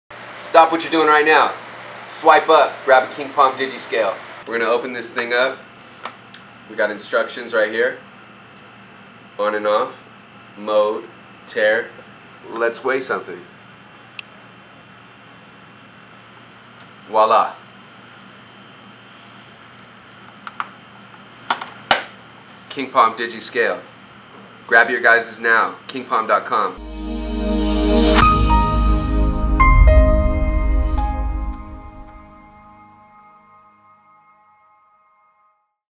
King Palm Digital Scale Product Weighs Up To 3 Ounces
The King Palm Scale allows you to weigh a maximum capacity of 100g and readabilities of 0.01g to guarantee you an accurate and precise weighing session. Our KP Scale is designed with a stainless steel platform and a protective flip cover. This pocket scale is lightweight and portable for easy transfer.

King Palm scale has easy-touch buttons, large-size digits, and a stark contrast LCD blue backlit display that makes it easy to read in all light conditions. The KP scale features four different weight modes: g / oz / ozt / dwt for easy weight translations, tare function for net determination, and a 60-second auto shut-off to preserve battery life.